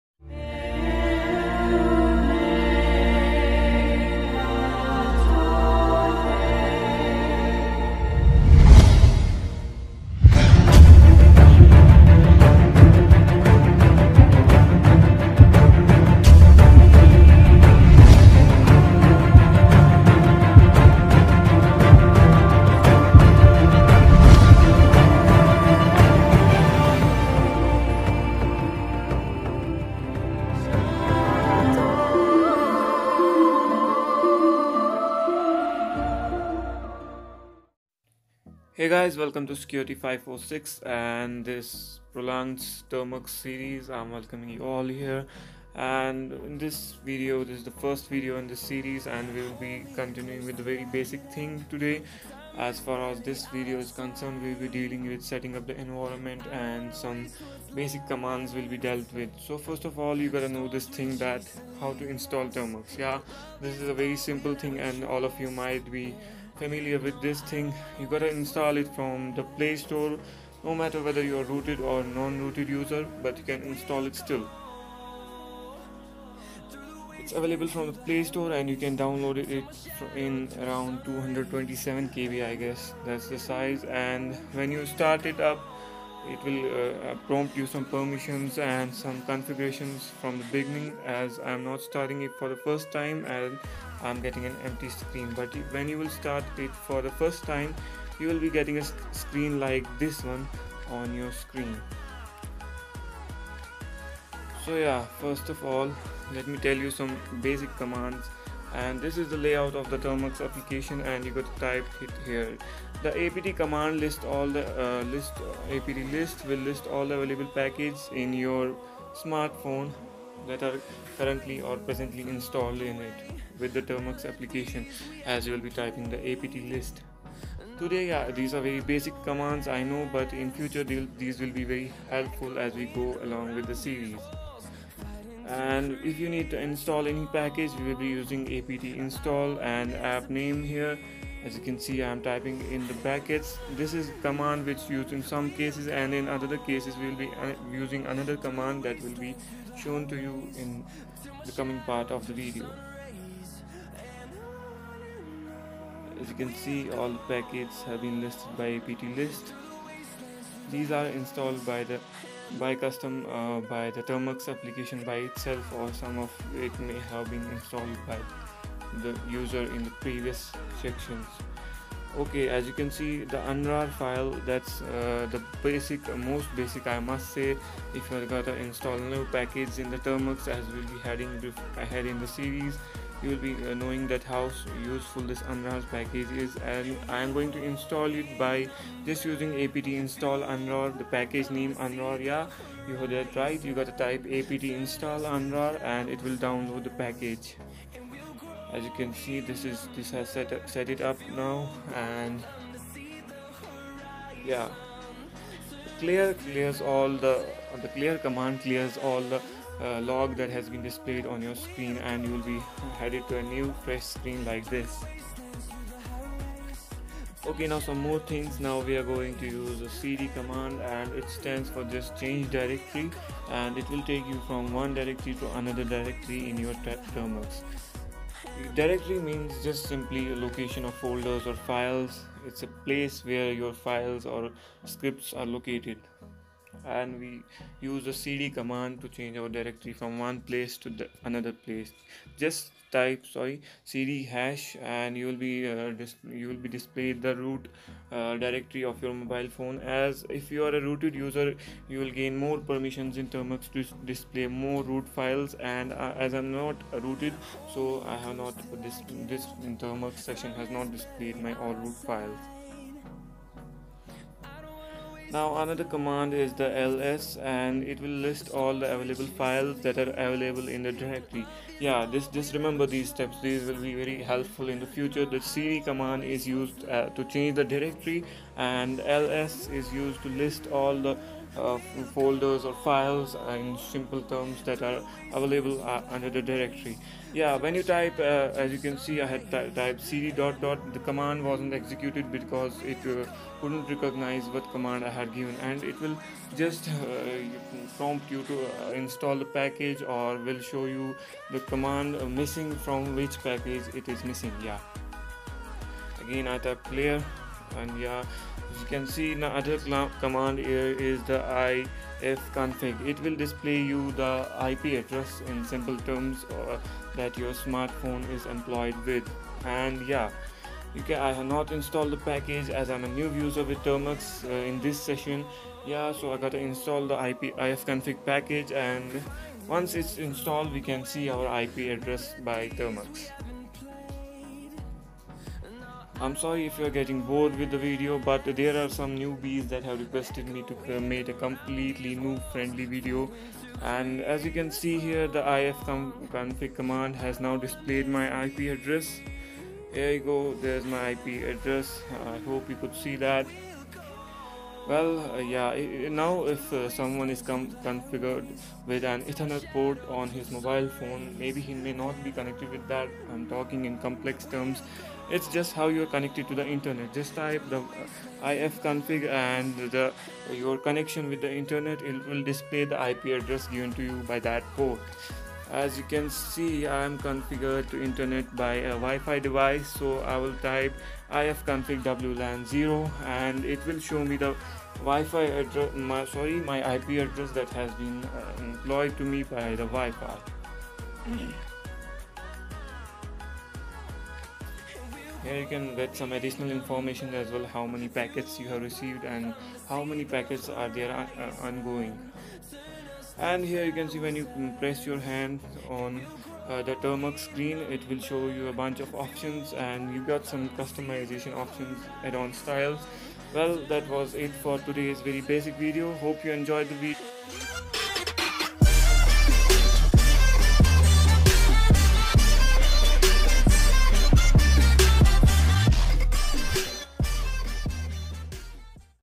Basics Of Termux -Lets Learn Termux - Part 01 || By Security 546
Welcome!!!
This is the most basic part in this series. Showing you some basic commands; this video also shows how to view ip address in termux. :)

IGNORE THESE TAGS PLEASE